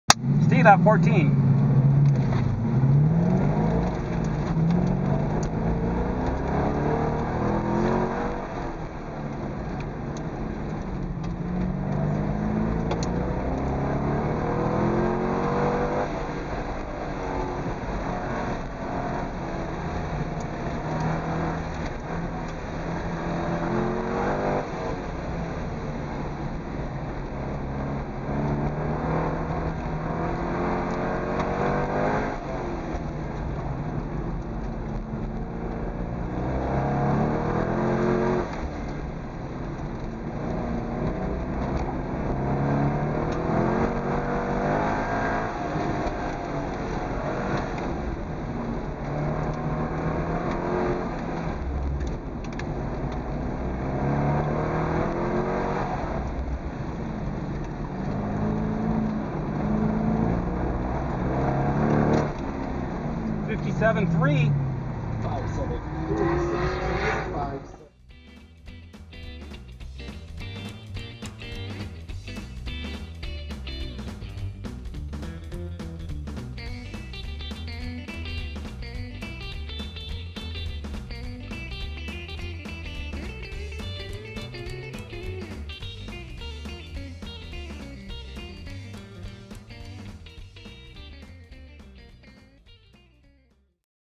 CSCC Autocross Practice. 5/23/2015.
4th. overall out of 70 cars.  Street tires.